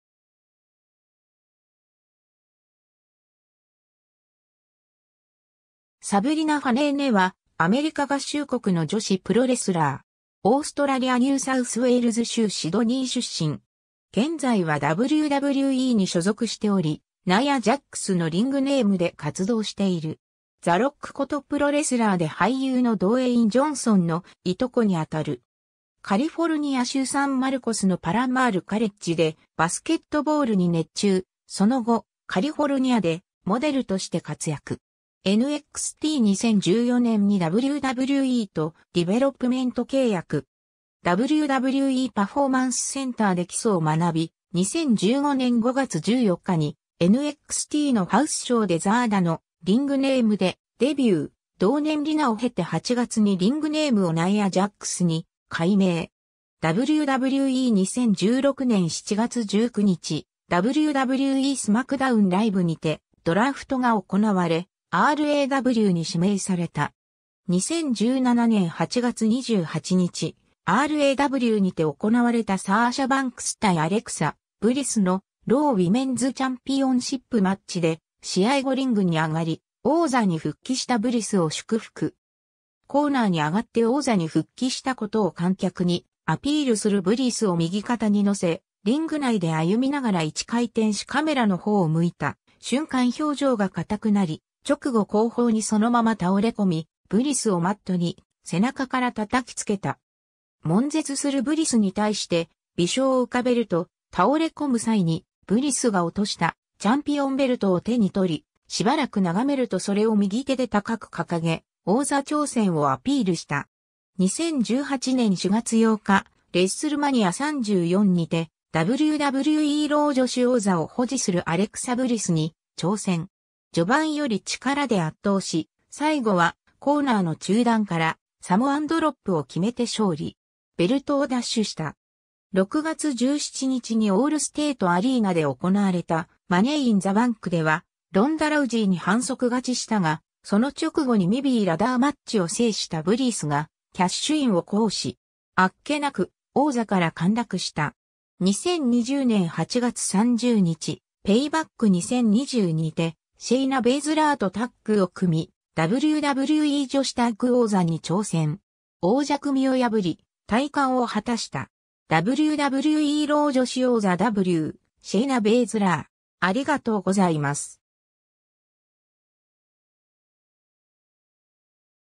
サブリナ・ファネーネ
サブリナ・ファネーネ（Savelina Fanene、1984年5月29日 - ）は、アメリカ合衆国の女子プロレスラー。
オーストラリアニューサウスウェールズ州シドニー出身。
現在はWWEに所属しており、ナイア・ジャックス（Nia Jax）のリングネームで活動している。
 ザ・ロックことプロレスラーで俳優のドウェイン・ジョンソンのいとこにあたる。
カリフォルニア州サンマルコスのPalomar Collegeでバスケットボールに熱中、その後、カリフォルニアでモデルとして活躍。
 NXT 2014年にWWEとディベロップメント契約。
WWEパフォーマンスセンターで基礎を学び、2015年5月14日にNXTのハウスショーでザーダのリングネームでデビュー、同年リナを経て8月にリングネームをナイア・ジャックス（Nia Jax）に改名。
 WWE 2016年7月19日、WWE・SmackDown LIVEにてドラフトが行われ、RAWに指名された。
 2017年8月28日、RAWにて行われたサーシャ・バンクス対アレクサ・ブリスの「Raw Women's Championship Match」で、試合後リングに上がり、王座に復帰したブリスを祝福。
コーナーに上がって王座に復帰した事を観客にアピールするブリスを右肩に乗せ、リング内で歩みながら一回転しカメラの方を向いた瞬間表情が固くなり、直後後方にそのまま倒れこみブリスをマットに背中から叩きつけた。
悶絶するブリスに対して微笑を浮かべると、倒れこむ際にブリスが落としたチャンピオンベルトを手に取り、しばらく眺めるとそれを右手で高く掲げ、王座挑戦をアピールした。
 2018年4月8日、レッスルマニア34にてWWEロウ女子王座を保持するアレクサ・ブリスに挑戦。
序盤より力で圧倒し、最後はコーナーの中段からサモアンドロップを決めて勝利。
ベルトを奪取した。
6月17日にオールステート・アリーナで行われたマネー・イン・ザ・バンクではロンダ・ラウジーに反則勝ちしたが、その直後にMITBラダーマッチを制したブリスがキャッシュインを行使。
あっけなく王座から陥落した。
 2020年8月30日、PayBack2020にて、シェイナ・ベイズラーとタッグを組みWWE女子タッグ王座に挑戦。
王者組(サーシャ・バンクス・ベイリー)を破り戴冠を果たした。
 WWEロウ女子王座 w /シェイナ・ベイズラー